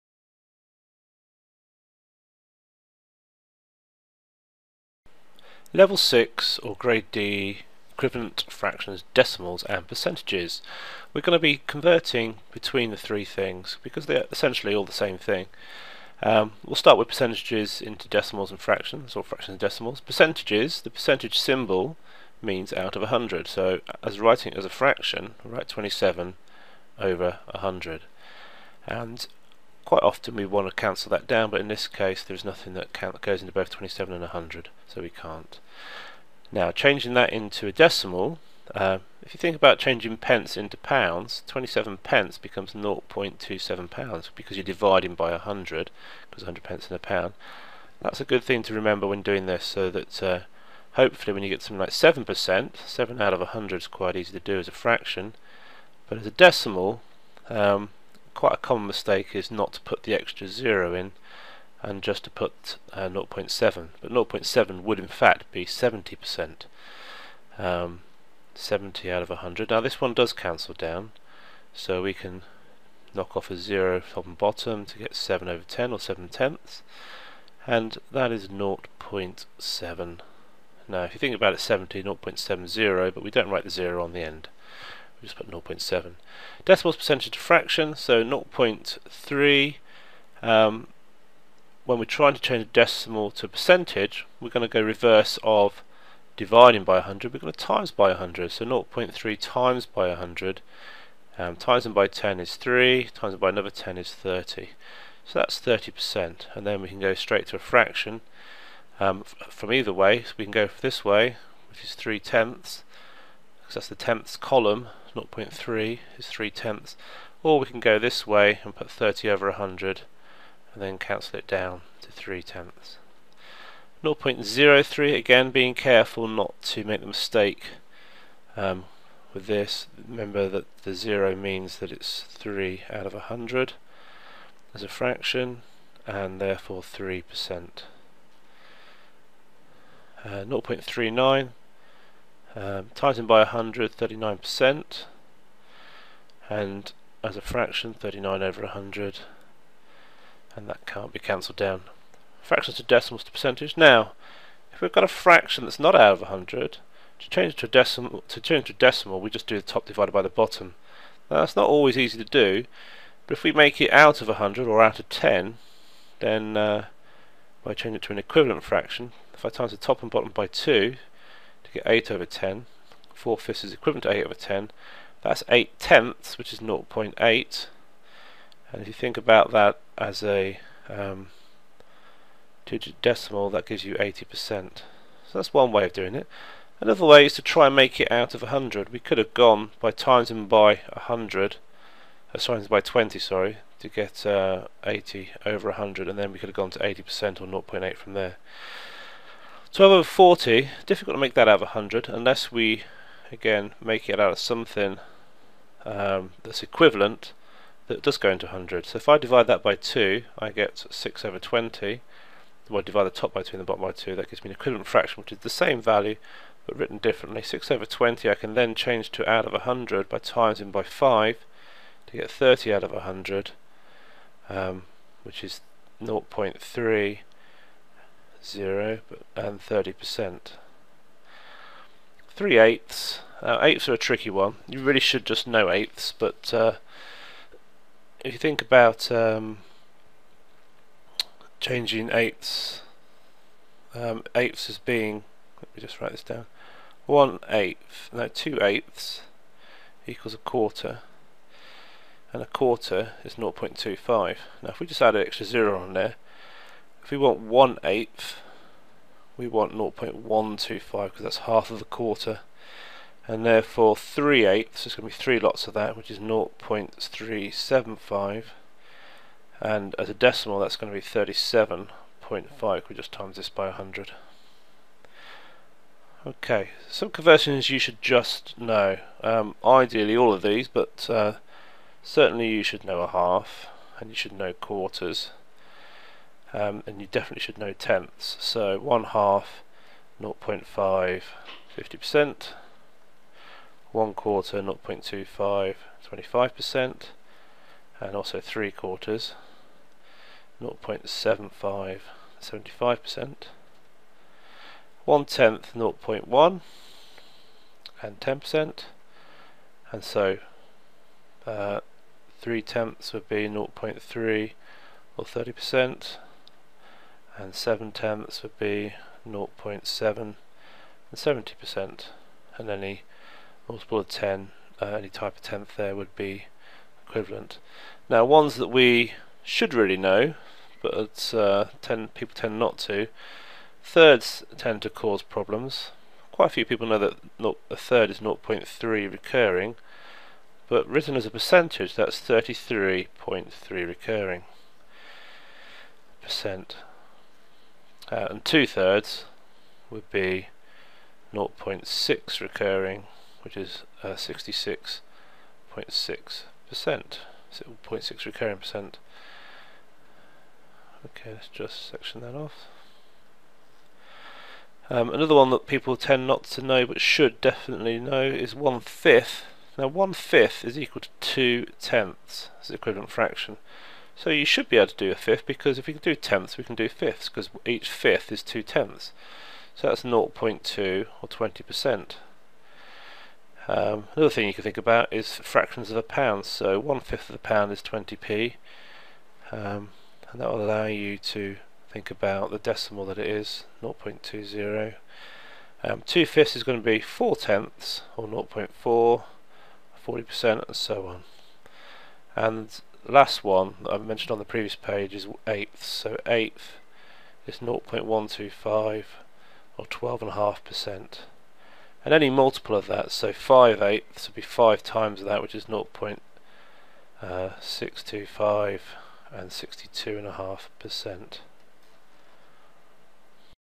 6.1 Equivalent Fractions, Decimals, Percentages-Basic Maths Core Skills Level 6/GCSE Grade D
Check out this playlist for all the videos covering level 6 basic maths skills
This video is part of a series that is linked to a specific system for improving Basic Maths skills. This one is a Level 6 Basic Maths Skills video for students trying to progress to level 7